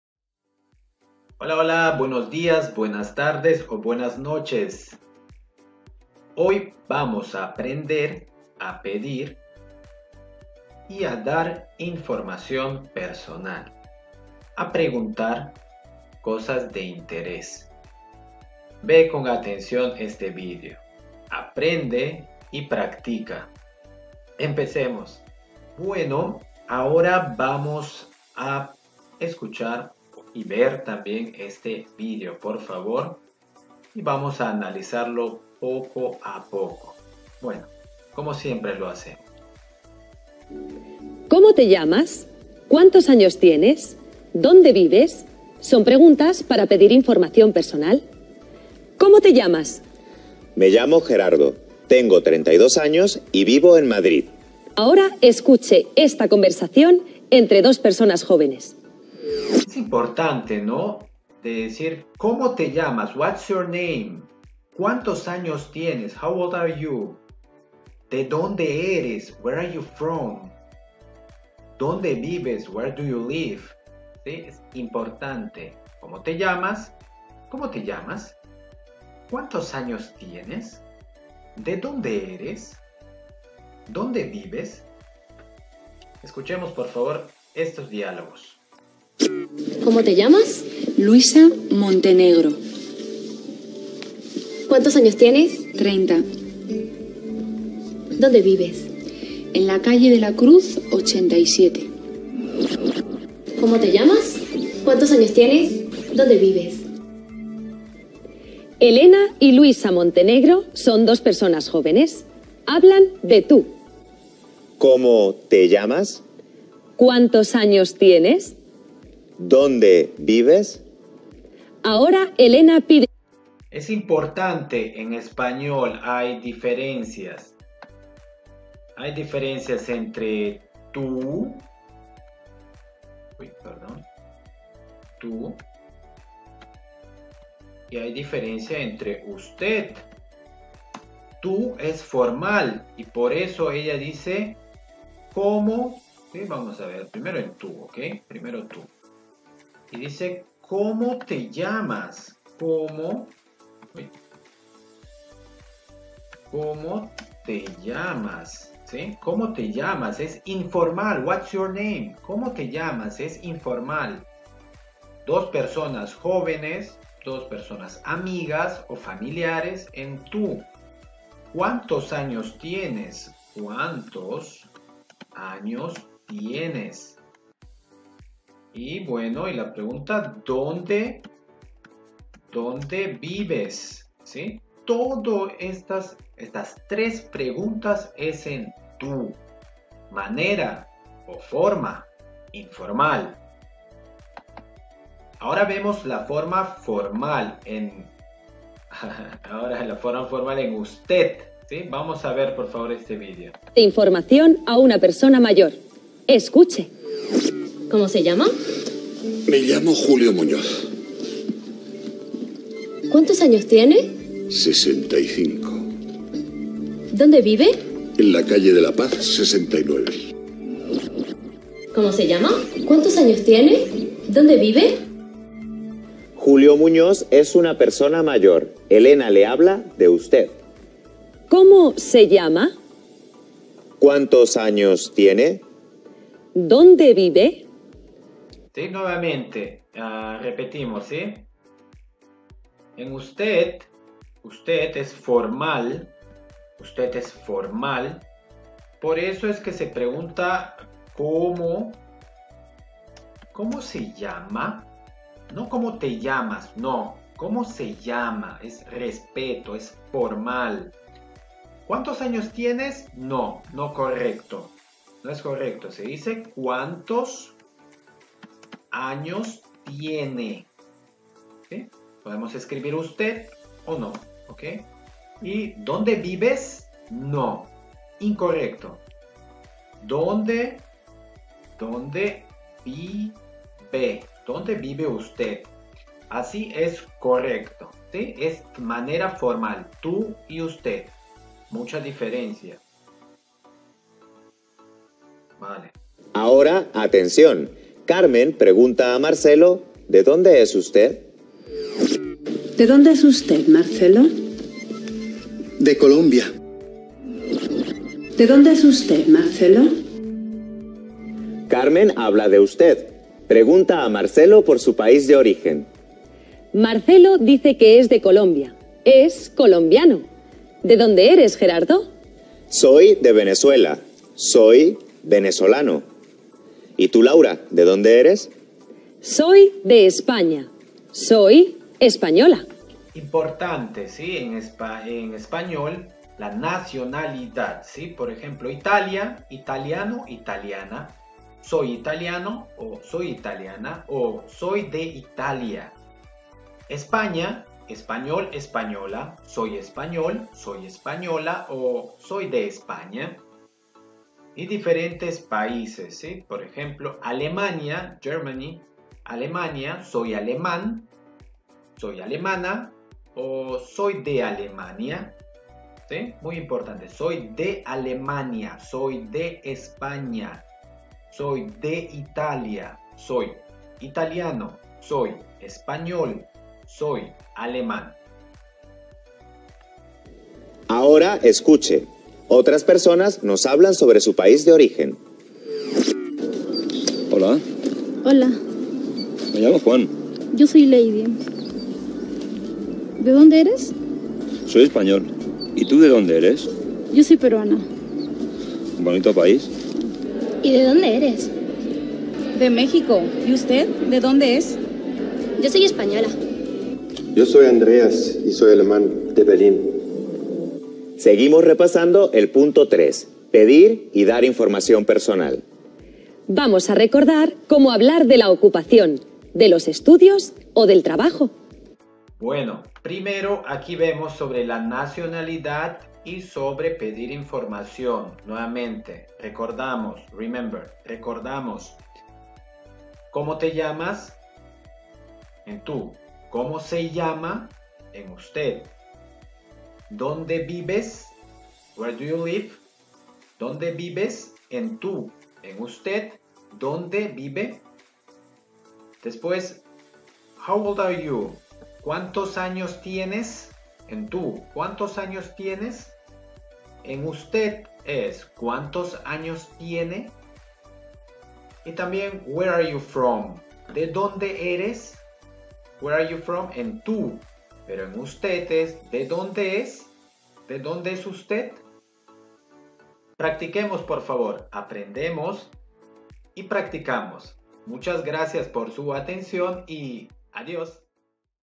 HOW TO ASK NAME/ NATIONALITY/ AGE/ IN SPANISH.
HOW TO ASK WHAT'S YOUR NAME/HOW OLD ARE YOU/ WHERE ARE YOU FROM/WHERE DO YOU LIVE/ IN SPANISH.